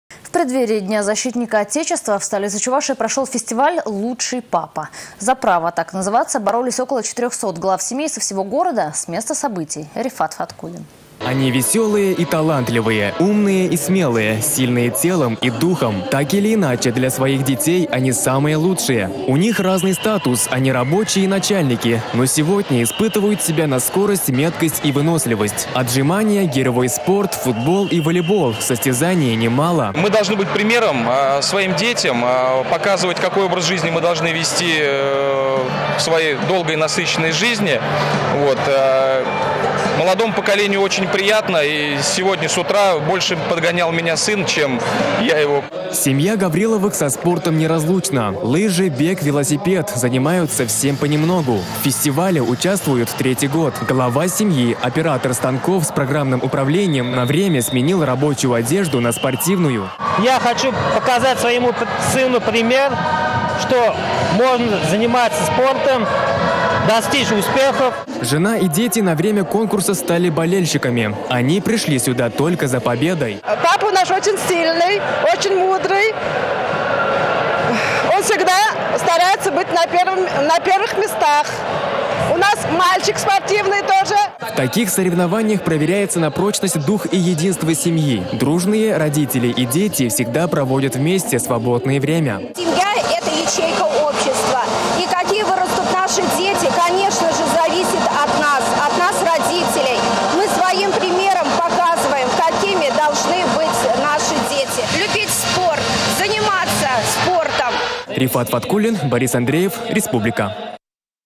В столице Чувашии прошёл фестиваль «Лучший папа».
За право  так называться боролись около 400 глав семей со всего города. С места событий  - Рифат Фаткуллин.